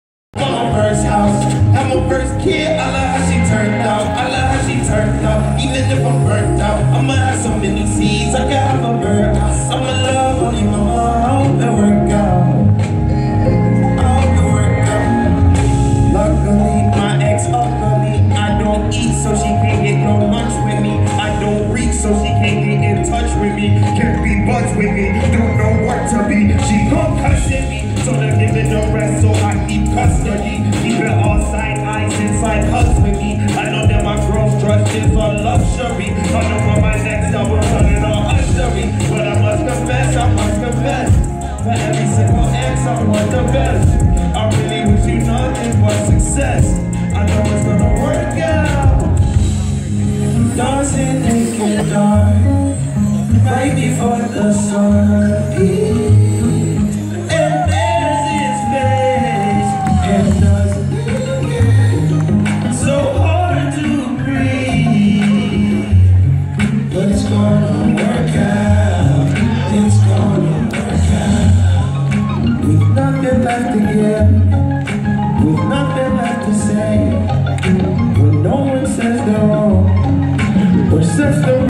Chance the Rapper live 10/31/2018 Greek theater
Chance the Rapper at the Mac Miller Celebration of life show